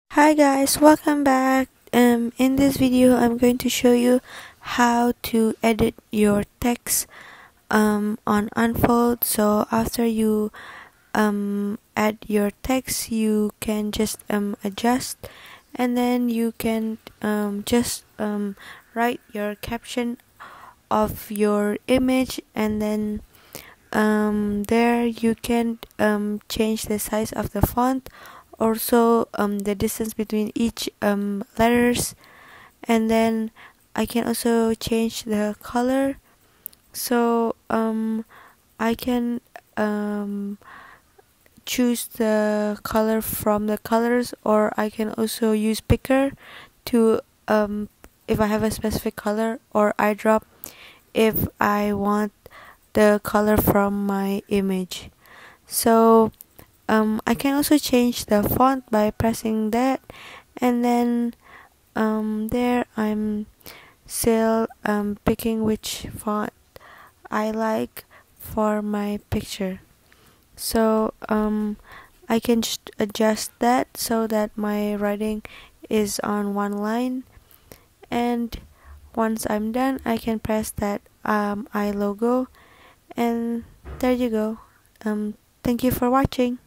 How To Add Text On Unfold
Hi guys! In this video I’ve shown you how to add, change fonts, change color and resize texts on Unfold